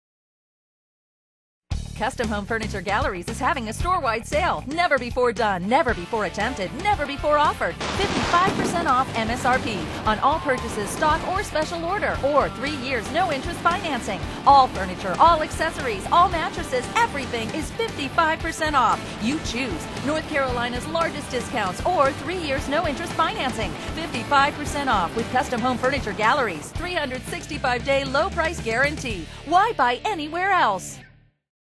Biggest Sale of the Year 2011/2012 - HELD OVER
Custom Home Furniture Galleries is holding over it's Biggest Sale of the Year. Don't miss out on North Carolina's largest discounts. This sale is for a limited time so hurry in. Manufacturers restrictions do apply.